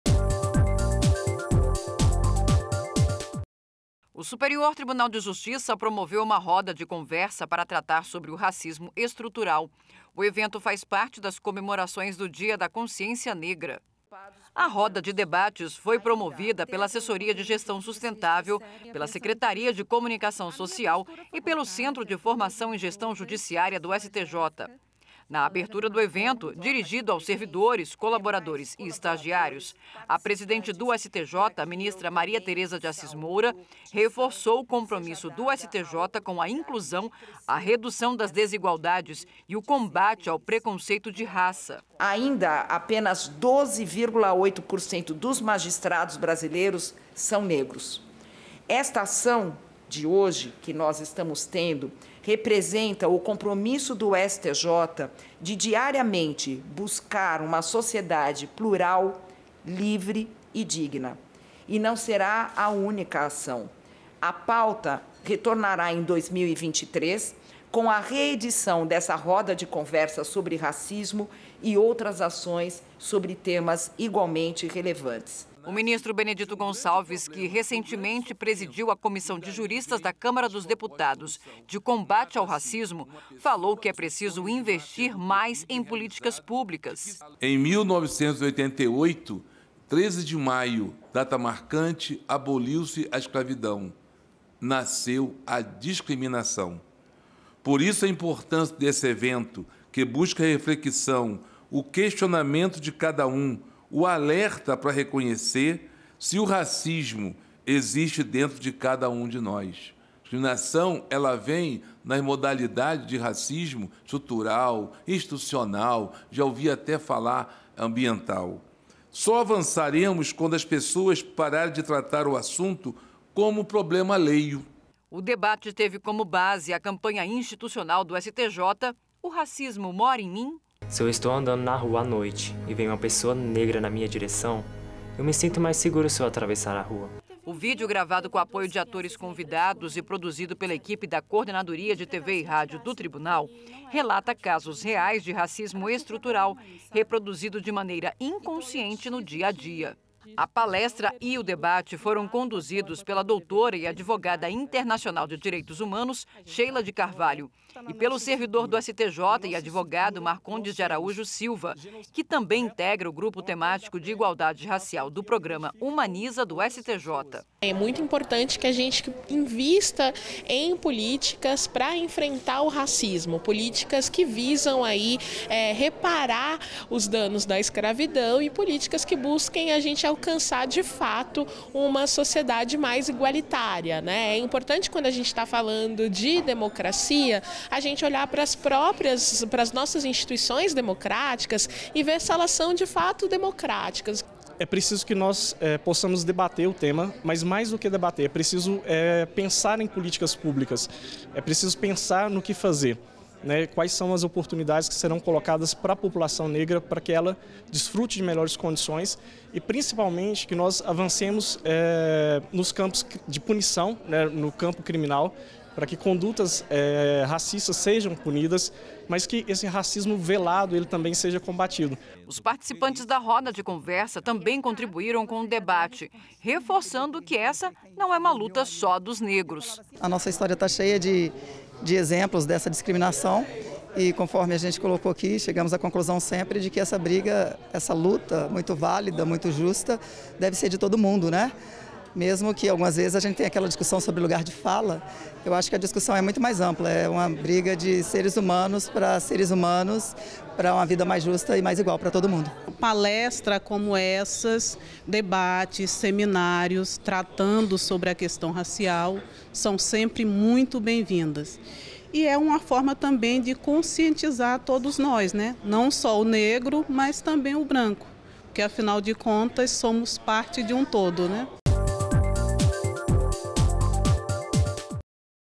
STJ promove roda de conversa sobre racismo estrutural 24.11.22
Para refletir e dialogar sobre a questão do racismo estrutural dentro e fora do Superior Tribunal de Justiça (STJ), foi promovida, nesta quarta-feira (23), a roda de conversa "O racismo mora em mim?". O evento contou com a participação de especialistas, ministros e de diversos servidores, estagiários e colaboradores da instituição.
Na abertura do evento, falaram a presidente do STJ, ministra Maria Thereza de Assis Moura, e o ministro Benedito Gonçalves, que atuou como presidente da comissão de juristas criada pela Câmara dos Deputados para aperfeiçoar a legislação contra o racismo. Ketlin Feitosa, assessora-chefe de Gestão Sustentável, mediou a conversa.